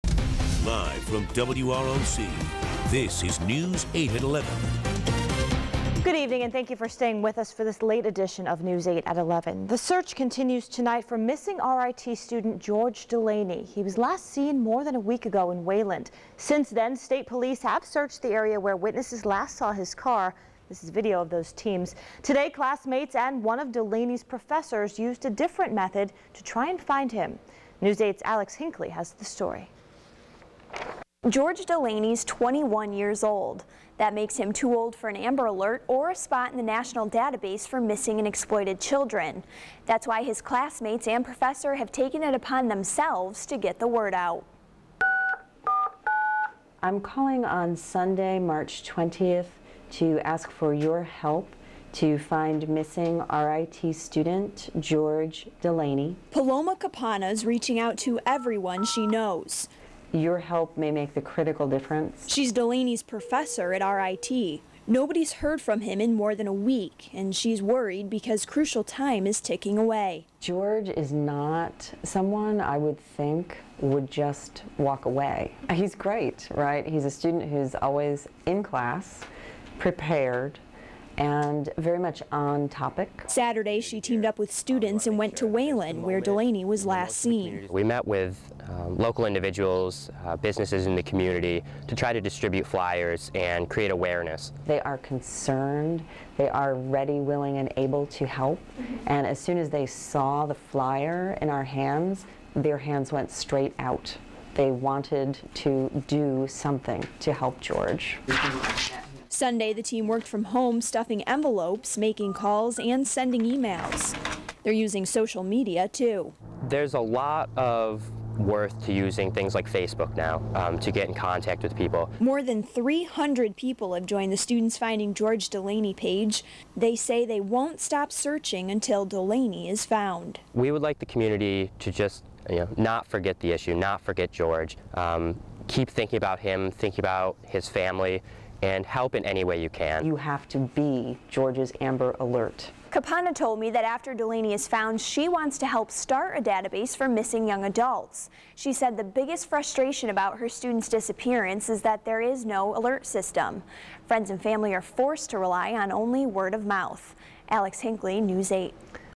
RIT on TV: Finding George Delany
WROC-TV (Channel 8) reports on efforts at Rochester Institute of Technology to search for missing student George Delany. Fellow RIT students have intensified their efforts to hand out fliers and spread the word through social media resources.